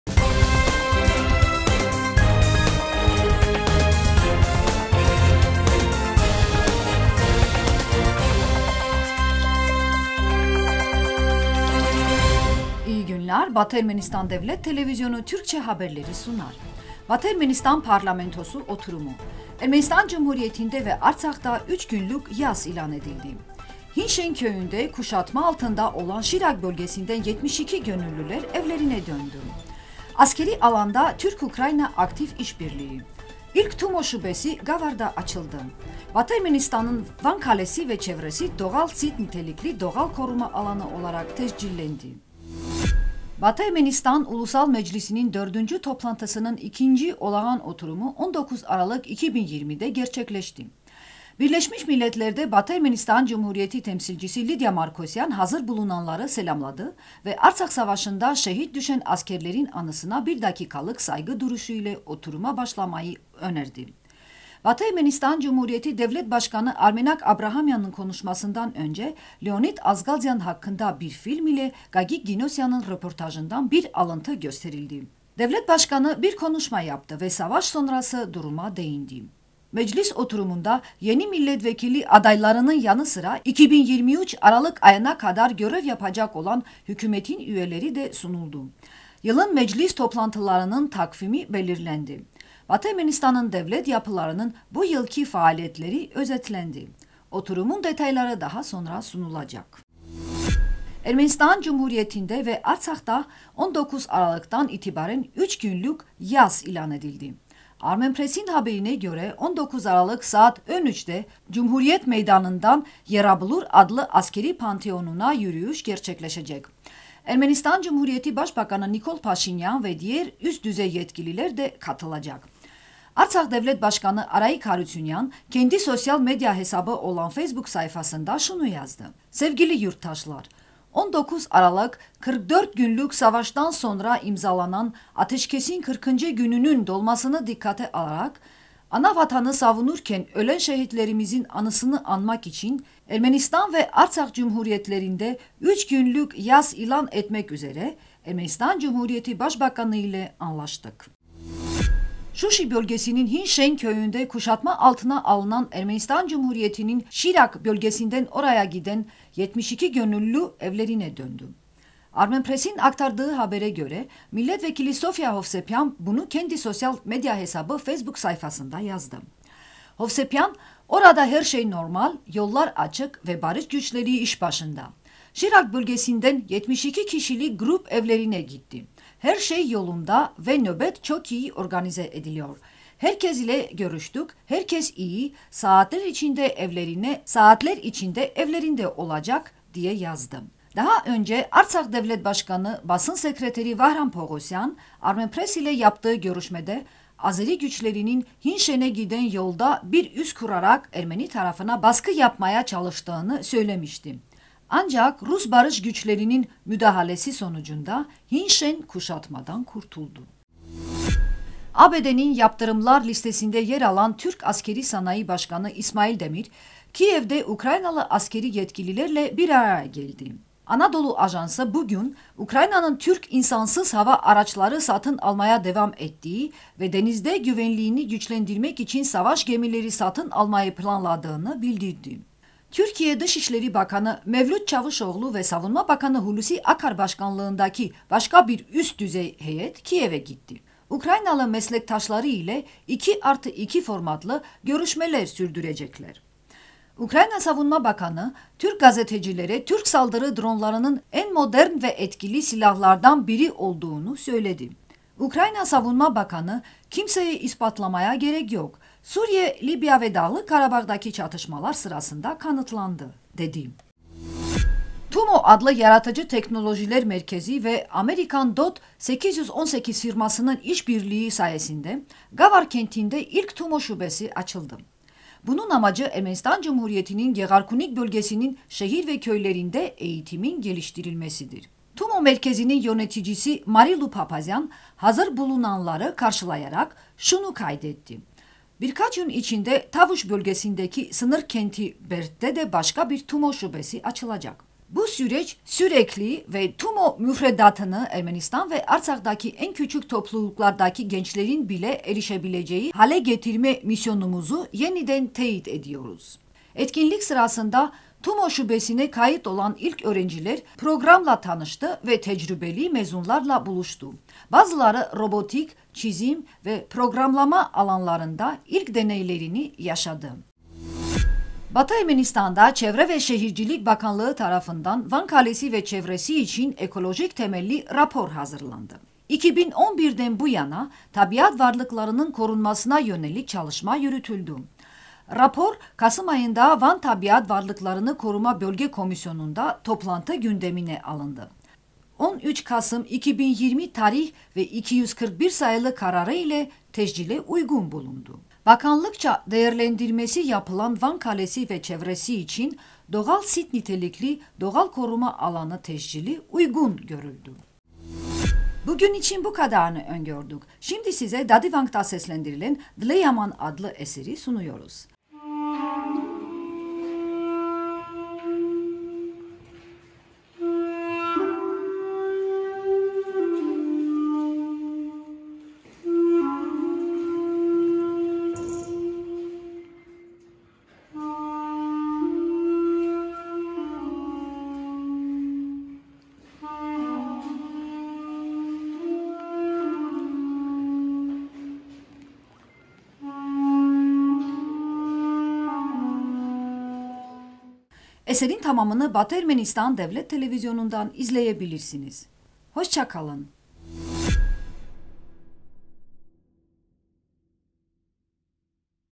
Batı Ermenistan Parlamentosu oturumu.Haberler 19.12.2020
1- Batı Ermenistan Parlamentosu oturumu
2- Ermenistan Cumhuriyeti’nde ve Artsakh'da üç günlük yas ilan edildi
3- Hin Şen köyünde kuşatma altında olan Şirak bölgesinden 72 gönüllüler evlerine döndü
5- İlk Tumo şubesi Gavar'da açıldı
6- Batı Ermenistan'ın Van Kalesi ve çevresi "doğal sit-nitelikli doğal koruma alanı" olarak tescillendi